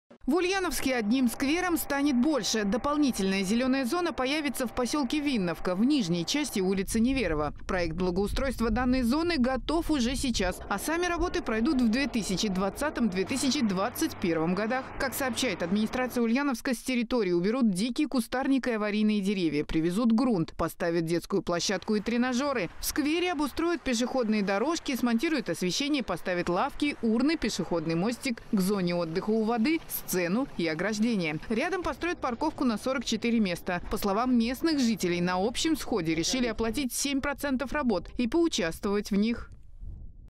Новый сквер
В Ульяновске одним сквером станет больше. Дополнительная зеленая зона появится в поселке Винновка в нижней части улицы Неверова. Проект благоустройства данной зоны готов уже сейчас а сами работы пройдут  в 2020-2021 годах.  Как сообщает администрация Ульяновска, с территории уберут дикий кустарник и аварийные деревья, привезут грунт, поставят детскую площадку и тренажеры. В сквере обустроят пешеходные дорожки, смонтируют освещение, поставят лавки, урны, пешеходный мостик к зоне отдыха у воды, сцену и ограждение. Рядом построят парковку на 44 места.  По словам местных жителей, на общем сходе решили оплатить 7% работ и поучаствовать в них.